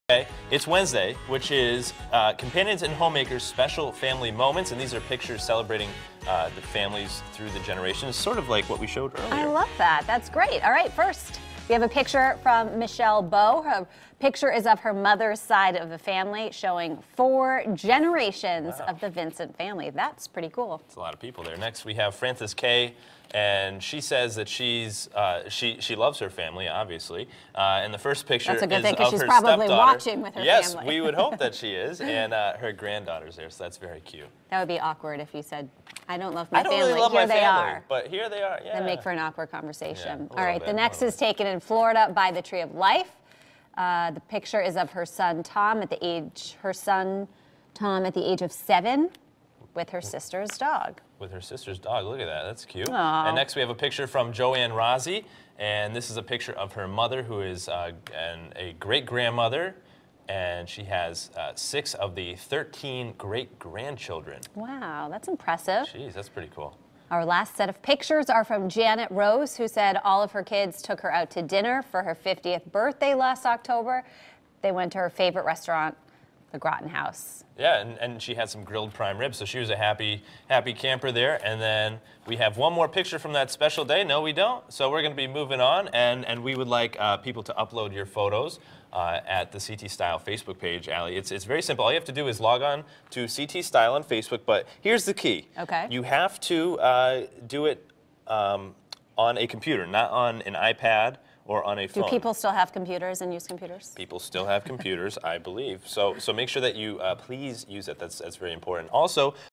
Companions & Homemakers’ Special Family Moments
To celebrate family generations- each week we are going to showcase family photos. Take a look at some special family photos in the video.